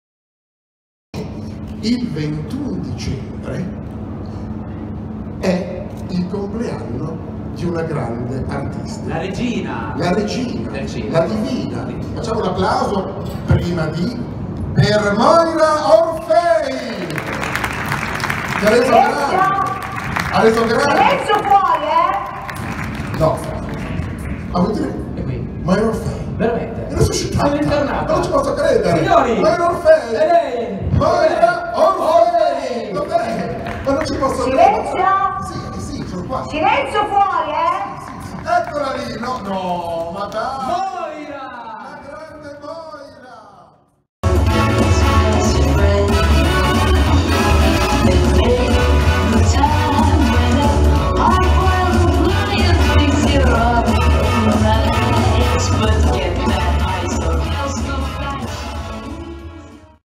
LA CESIRA & CIRCUS 5 - 21 DICEMBRE 2017
Un evento unico.. per la prima volta una serata al Circo di Nando Orfei a Milano Idroscalo dove si sono esibiti gli artisti del Circo e lo staff del Dq Milano, cabaret con Eraldo Moretto La Cesira, lo staff DQ e le Drag Queen.. un evento ricco di emozioni ! ! !